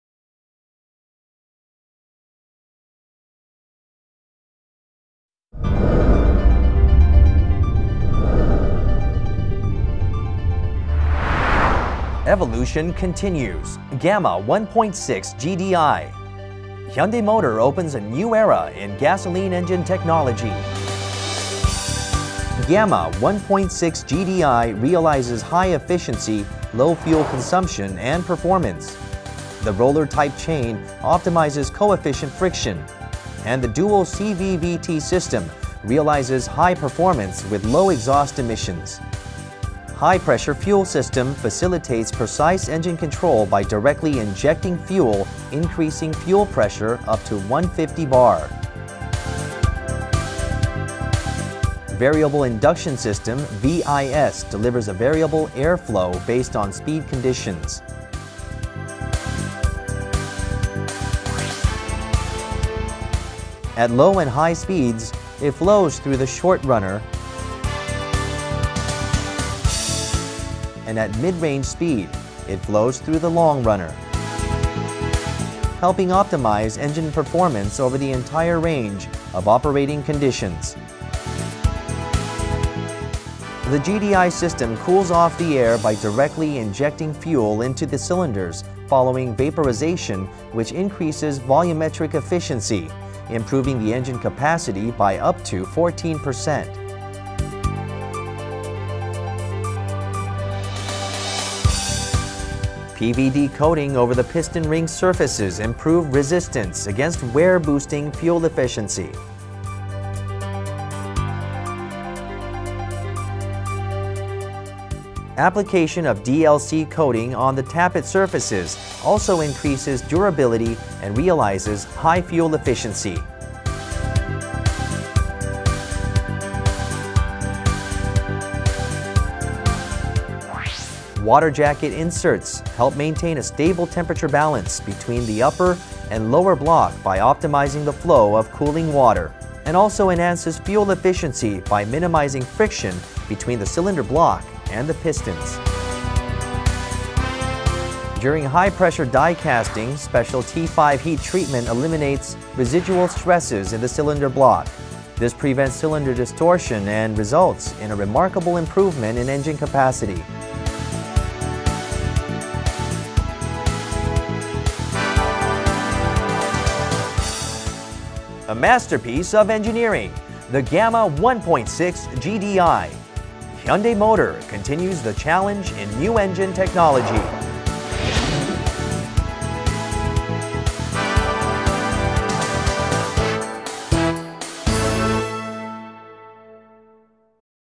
Hyundai's New Gamma GDI Engine
Hyundai Motor Co. unveiled its all-new Gamma GDI engine. It is the smallest Hyundai engine yet to benefit from the application of Gasoline Direct Injection technology.  The new Gamma also features Dual Continuously Variable Valve Timing, a Variable Induction System and exotic anti-friction coatings such as CrN Physical Vapor Deposition (PVD) coating and Diamond Like Carbon (DLC) coating.